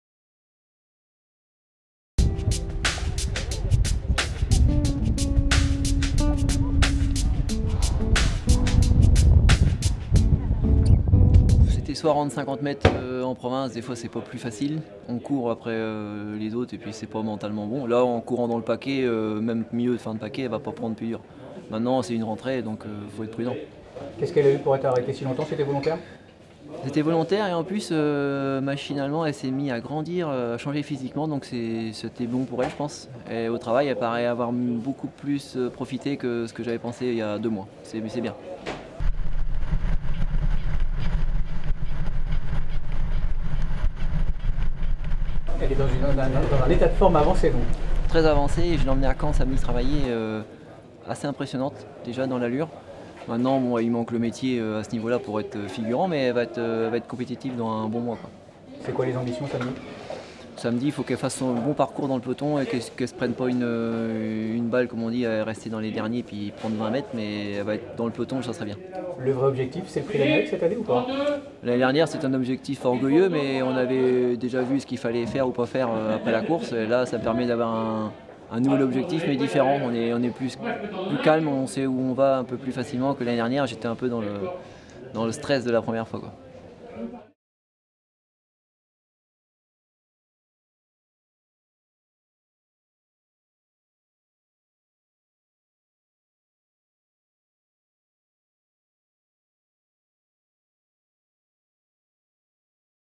VANIKA DU RUEL effectue sa rentrée dans le Prix de Bretagne. Franck ANNE nous en parle - A2TURF.COM
La championne de Franck ANNE, VANIKA DU RUEL, effectue sa rentrée à l'occasion du Prix de Bretagne 2013.
Son mentor nous donne de ses nouvelles et nous explique ses ambitions pour samedi ainsi que pour la suite de l'hiver.